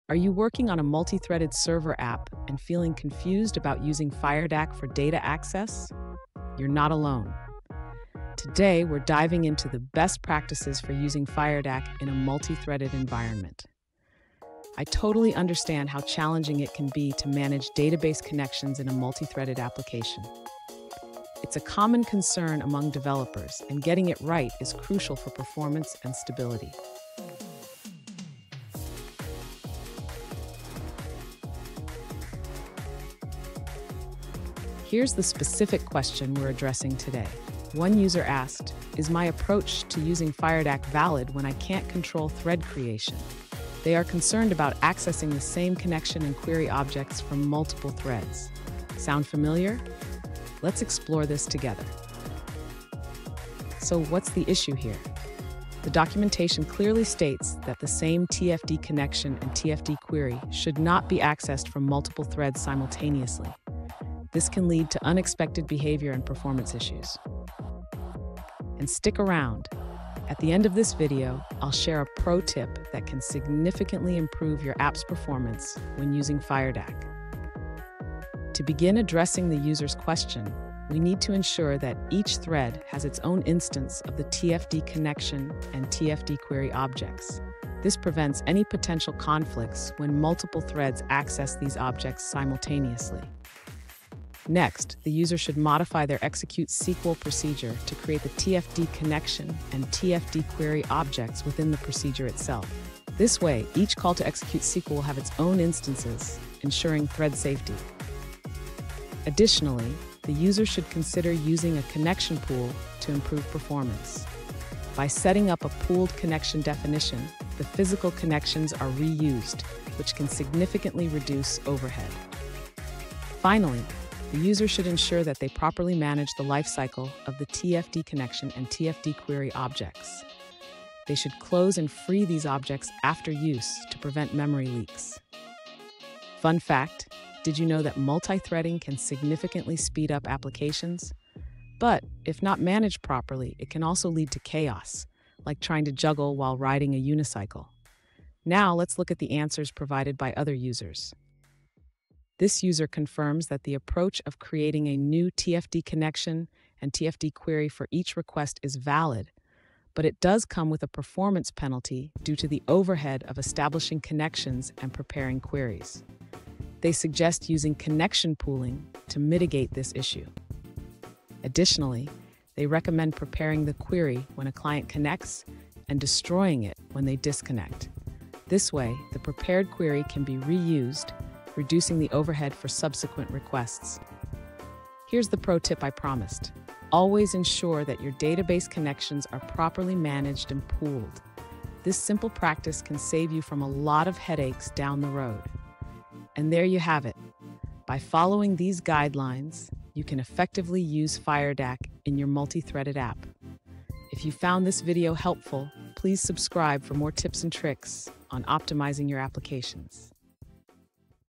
Firedac Multithreading Issues: Solutions for Delphi Developers
In this video, we dive into the common multithreading issues faced by Delphi developers when using Firedac. As applications become more complex and performance-driven, understanding how to effectively manage database connections and operations in a multithreaded environment is crucial. Join us as we explore practical solutions and best practices to enhance your development experience and ensure smooth, efficient database interactions.

Today's Topic: Firedac Multithreading Issues: Solutions for Delphi Developers

Related to